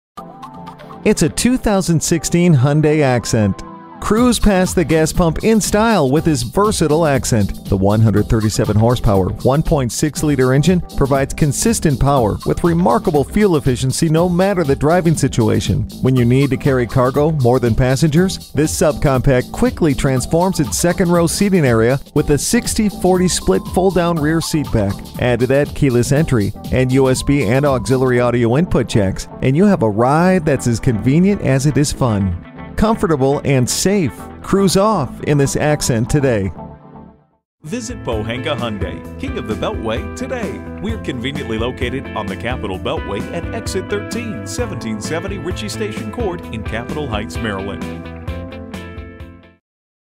2016 Hyundai Accent Capitol Heights MD Washington-DC, MD FGU237083 - SOLD
Year: 2016
Make: Hyundai
Model: Accent
Trim: SE
Engine: 1.6L I4 DGI DOHC 16V
Transmission: 6-Speed Automatic with Shiftronic
Color: Red
Stock #: FGU237083
VIN: KMHCT5AE4GU237083

Address:
1770 Ritchie Station Court